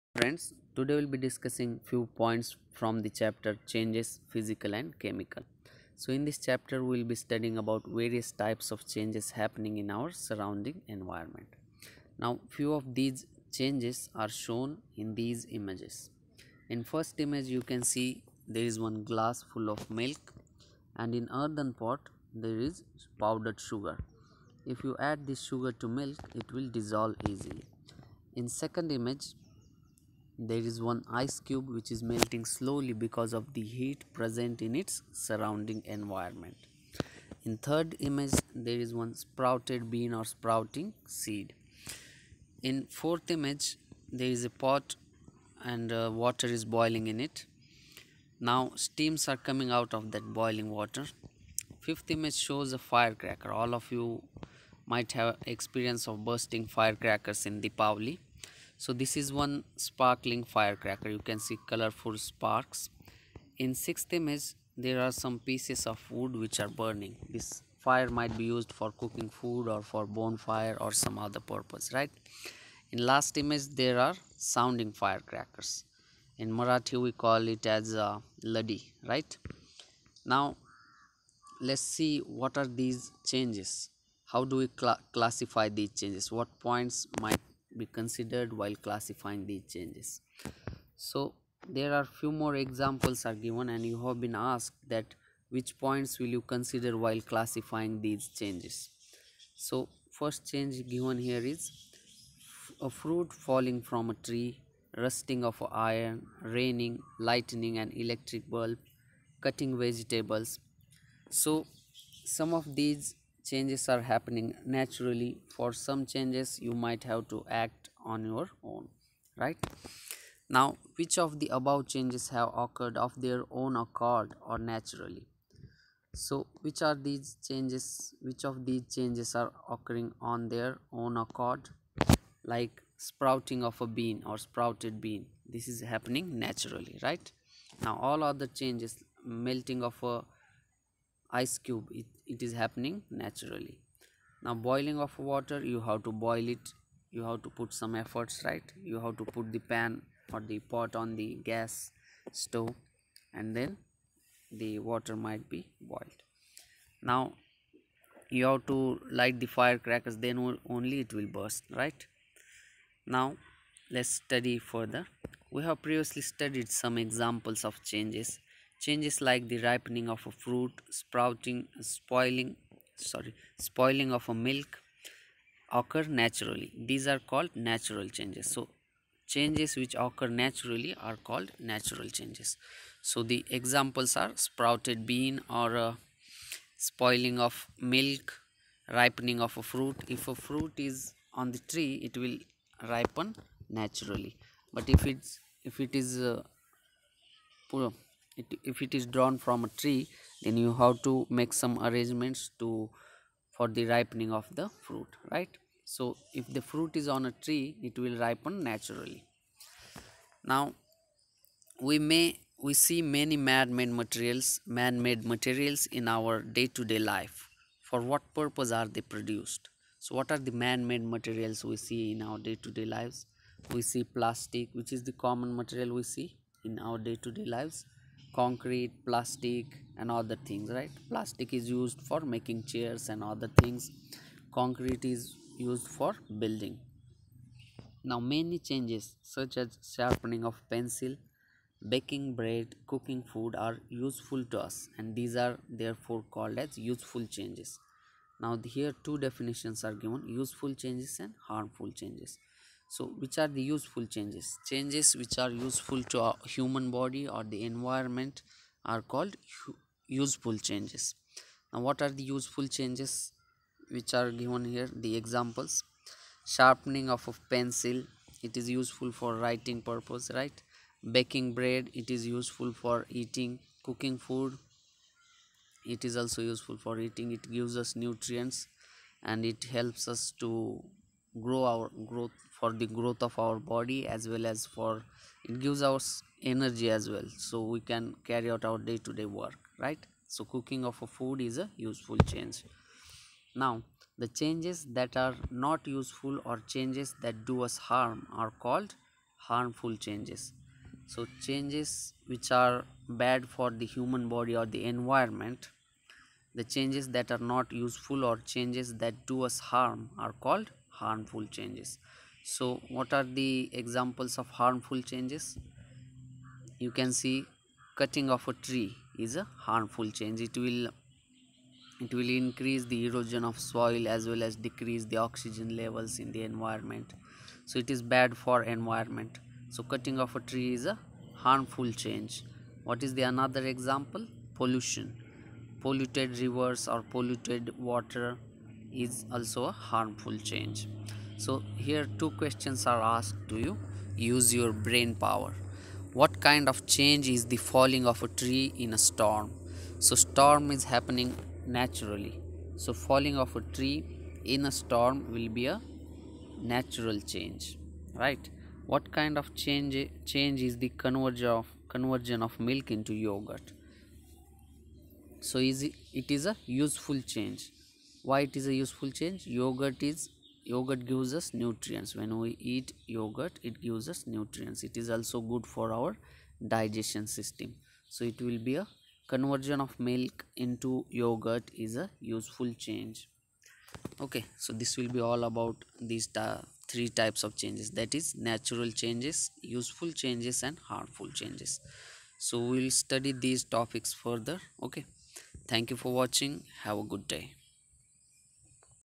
Natural and Useful Changes, Science, Class 7,Maharashtra,Chapter 13: Changes – Physical and Chemical
Natural changes - changes which happen mainly due to the natural factors with humans having no control over it.
Ripening
of a fruit, spoiling of milk occur naturally.

Useful changes - Many changes such as sharpening
a pencil, baking bread, cooking food
are useful to us and these are,
therefore, called useful changes.

Harmful changes - The
changes that are not useful or
changes that do us harm are called
harmful changes.

Note : Please watch the video's from ' Practical ' playlist for practical examples.

Chapter 13: Changes – Physical and Chemical, Natural, Useful and Harmful Changes, Science, Class 7, Maharashtra board